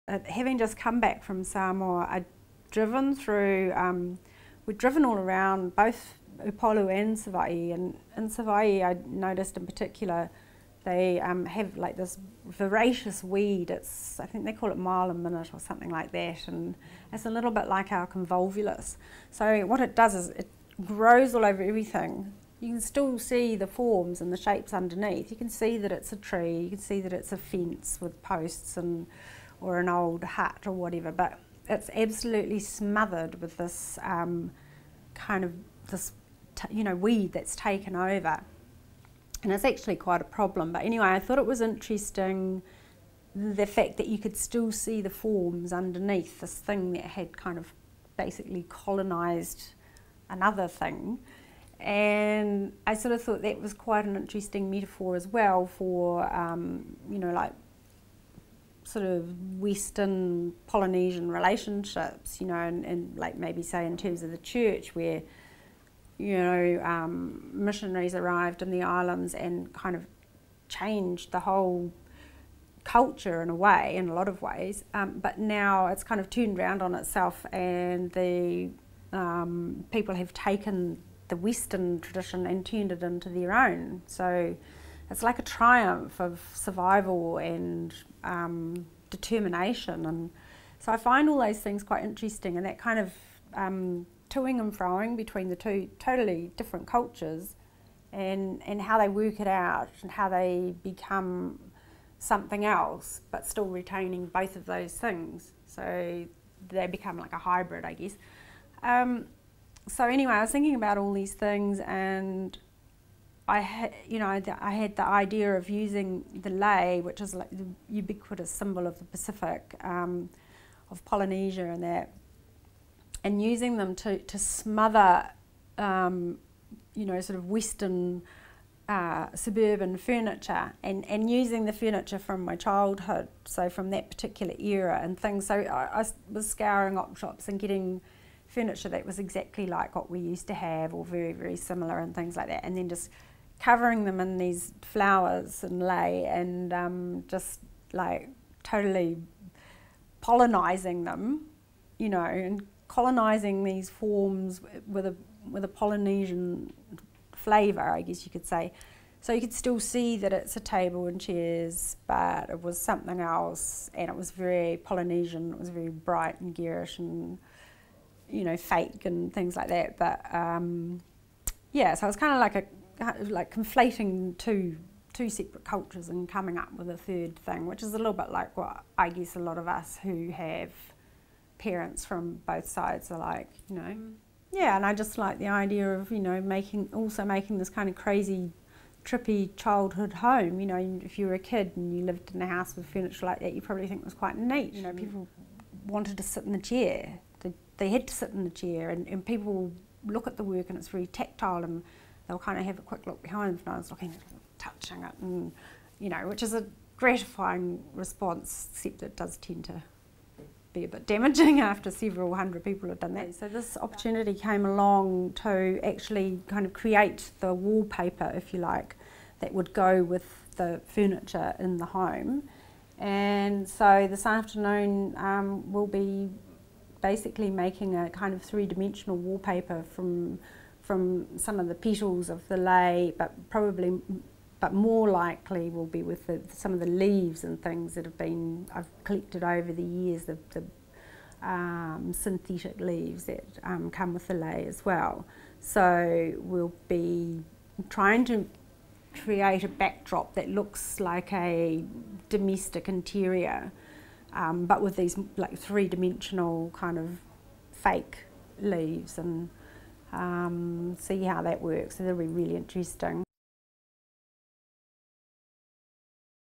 In Flyte
In Flyte surveys highlights from the past fifteen years of Niki Hastings-McFall's creative practice. The exhibition showcases the full scope of her art practice from her seminal lei and light box series to her more recent diorama and lamp series.

Cross-cultural translation has been a central concern throughout Hastings-McFall's artistic career. Her work is essentially urban and biographical and is shaped by her research into, and encounters with, her dual Samoan and pakeha heritage. Born in Titirangi, Auckland, Hastings-McFall was raised by her European grandparents and first met her Samoan father in 1992.

A key figure in the contemporary Pacific art scene Hastings-McFall's work draws attention to the cultural stereotypes of the Pacific within contemporary culture, including the influence of Christianity in Samoan culture.